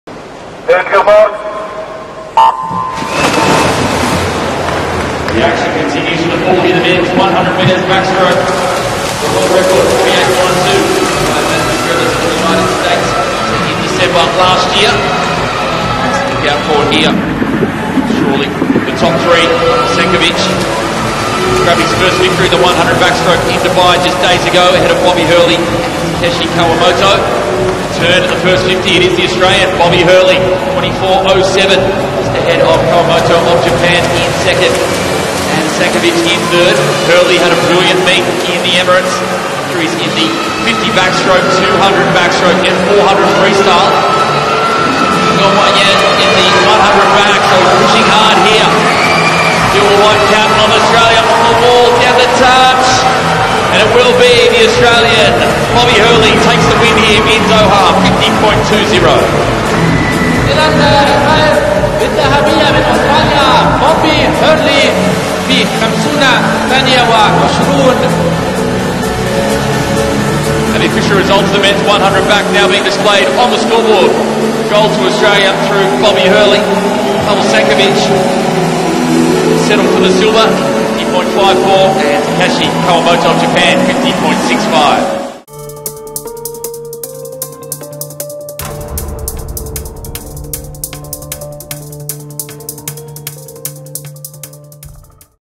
2nd title for Hurley after Paris - 100m Backstroke #6 Doha
At the sixth leg of the FINA/airweave Swimming World Cup in Doha Bobby Hurley (AUS) won his second title this season in the 100m back after Paris-Chartres.